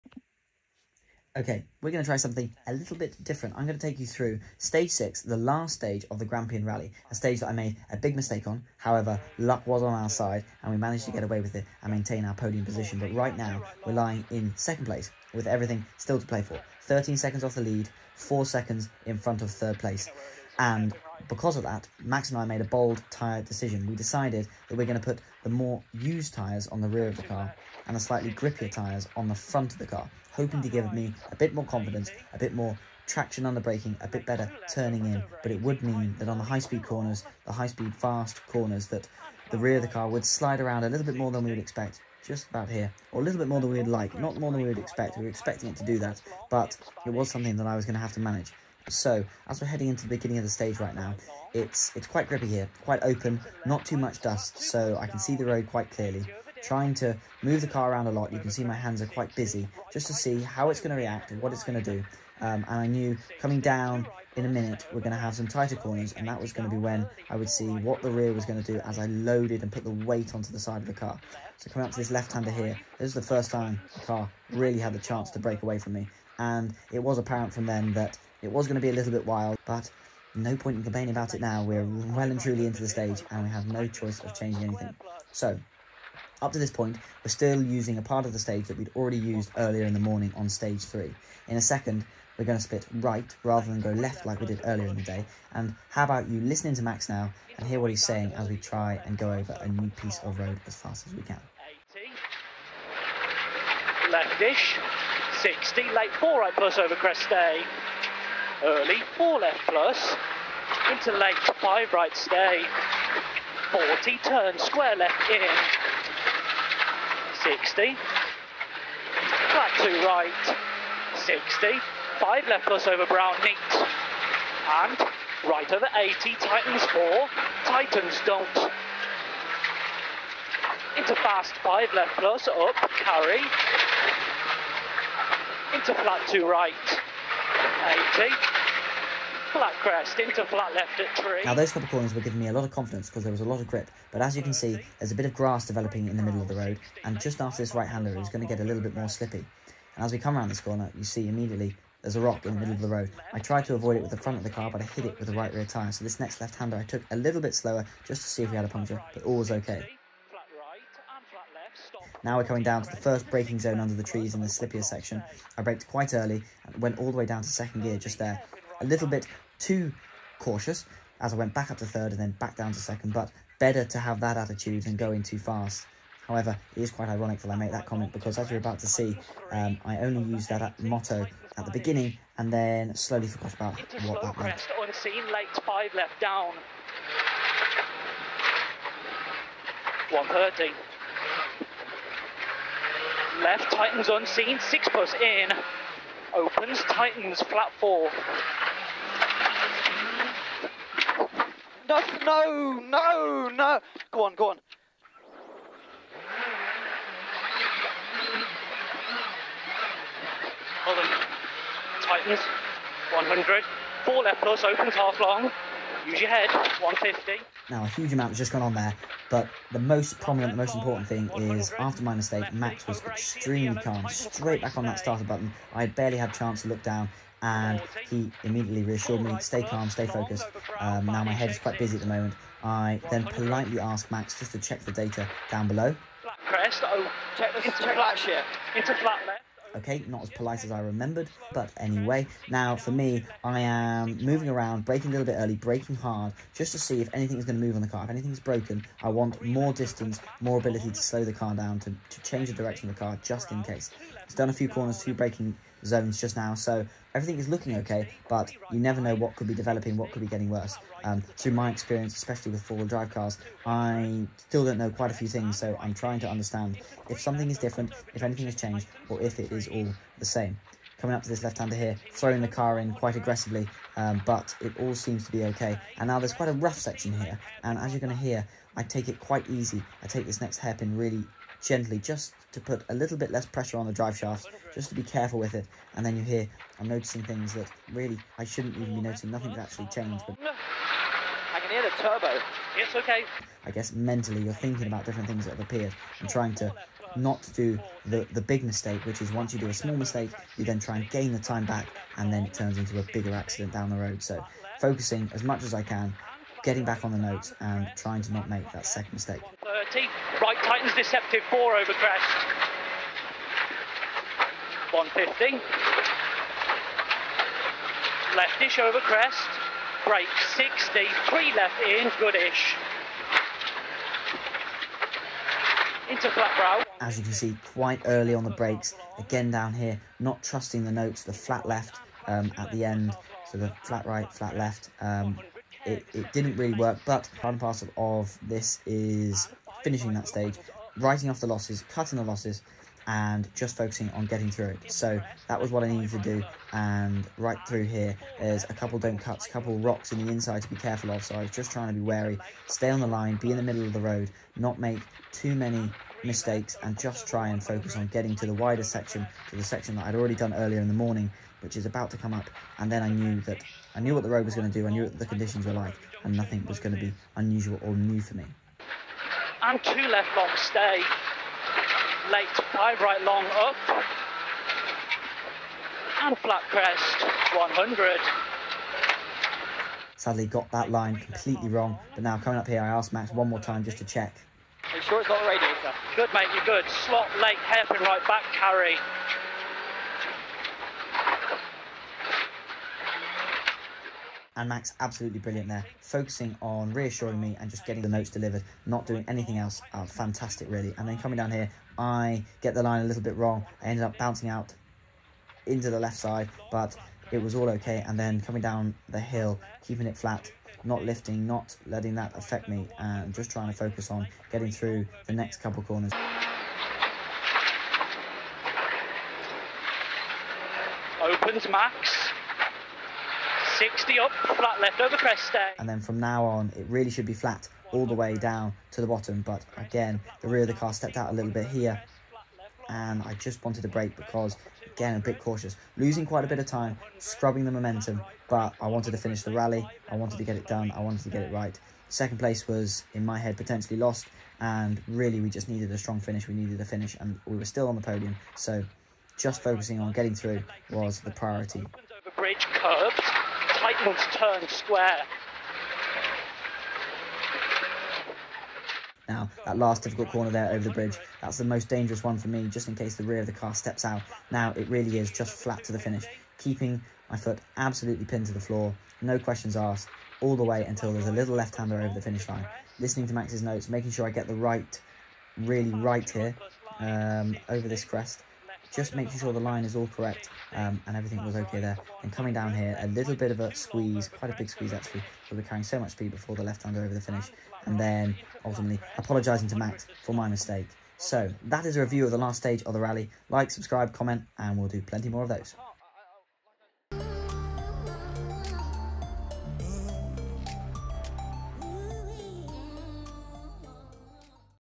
Stage 6 Hurlie Bog Grampian Rally (HUGE MOMENT HITTING TREE) - Drivers Cut
We're trying something a little different with a driver review of the last stage of the Grampian Rally.

Jump Onboard as I take you through the stage and give you a small glimpse into my thoughts and how we tackled the stage.